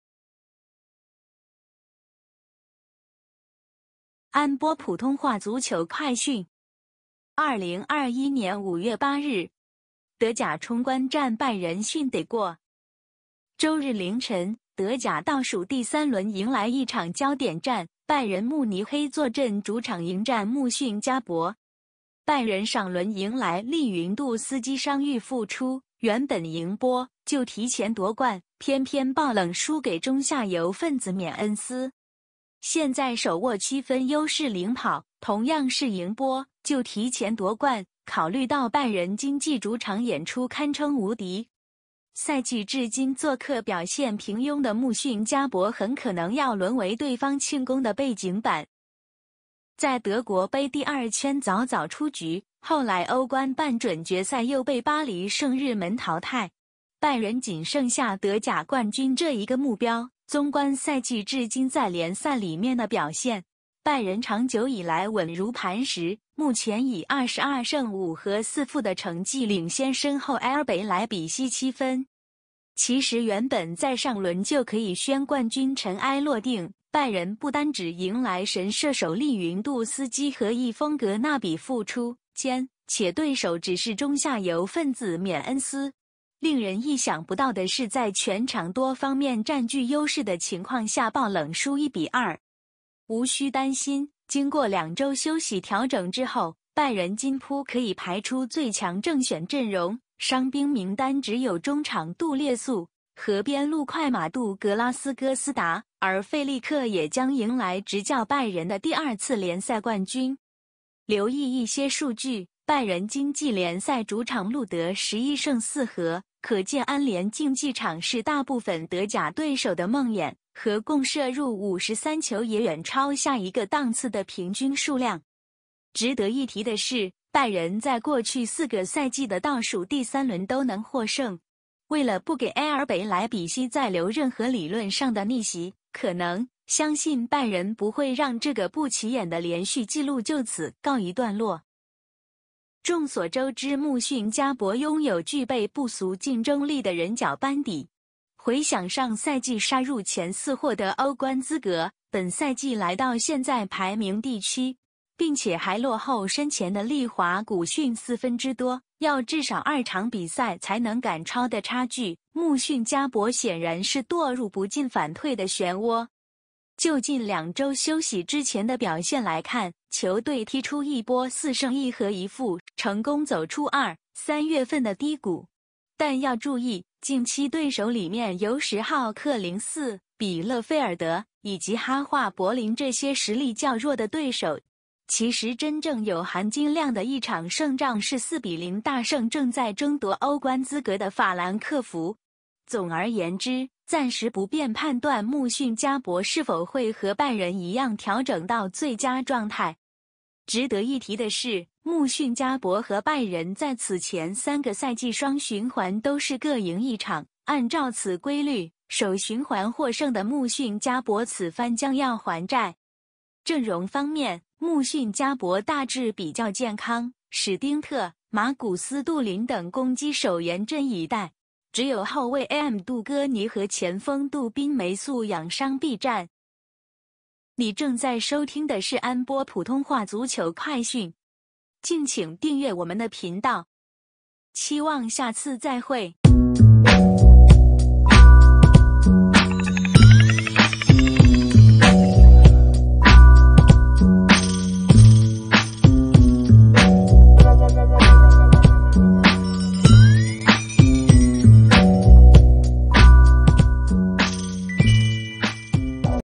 (普)【德甲-賽前新聞】2021-05-09 拜仁慕尼黑 VS 慕遜加柏 | 德甲衝冠戰拜仁「遜」得過
週日凌晨，德甲 倒數第三輪迎來一場焦點戰，拜仁慕尼黑 坐鎮主場迎戰 慕遜加柏 。拜仁上輪迎來利雲度斯基傷癒復出，原本贏波就提前奪冠，偏偏爆冷輸給中下游份子緬恩斯。現在手握7分優勢領跑，同樣是贏波就提前奪冠，考慮到拜仁今季主場演出堪稱無敵，賽季至今作客表現平庸的慕遜加柏很可能要淪為對方慶功的背景板。

在德國盃第二圈早早出局，後來歐冠半準決賽又被巴黎聖日門淘汰，拜仁僅剩下德甲冠軍這一個目標，綜觀賽季至今在聯賽裡面的表現，拜仁長久以來穩如磐石，目前以22勝5和4負的成績領先身後RB萊比錫7分。其實原本在上輪就可以宣佈冠軍塵埃落定，拜仁不單止迎來神射手利雲度斯基和翼鋒格納比復出，兼且對手只是中下游分子緬恩斯，令人意想不到的是在全場多方面佔據優勢的情況下爆冷輸1比2。無需擔心，經過兩週休息調整之後，拜仁今鋪可以排出最強正選陣容，傷兵名單只有中場杜列素和邊路快馬杜格拉斯哥斯達，而費歷克也將迎來執教拜仁的第二次聯賽冠軍。留意一些數據，拜仁今季聯賽主場錄得11勝4和，可見安聯競技場是大部分德甲對手的夢魘，合共射入53球也遠超下一個檔次的平均數量。值得一提的是，拜仁在過去4個賽季的倒數第三輪都能獲勝，為了不給RB萊比錫再留任何理論上的逆襲可能，相信拜仁不會讓這個不起眼的連續紀錄就此告一段落。

眾所周知慕遜加柏擁有具備不俗競爭力的人腳班底，回想上賽季殺入前四獲得歐冠資格，本賽季來到現在排名第七並且還落後身前的利華古遜4分之多，要至少2場比賽才能趕超的差距，慕遜加柏顯然是墮入不進反退的漩渦。就近兩週休息之前的表現來看，球隊踢出一波4勝1和1負，成功走出二、三月份的低谷；但要注意，近期對手裡面有史浩克零四、比勒費爾德以及哈化柏林這些實力較弱的對手，其實真正有含金量的一場勝仗是4比0大勝正在爭奪歐冠資格的法蘭克福，總而言之，暫時不便判斷慕遜加柏是否會和拜仁一樣調整到最佳狀態。值得一提的是，慕遜加柏和拜仁在此前3個賽季雙循環都是各贏1場，按照此規律，首循環獲勝的慕遜加柏此番將要「還債」。陣容方面，慕遜加柏大致比較健康，史丁特、馬古斯杜林等攻擊手嚴陣以待，只有後衛M.杜哥尼和前鋒杜賓梅素養傷避戰。

►單場賽事低至MOP$15, 立即下載M直播觀看#英超 精彩賽事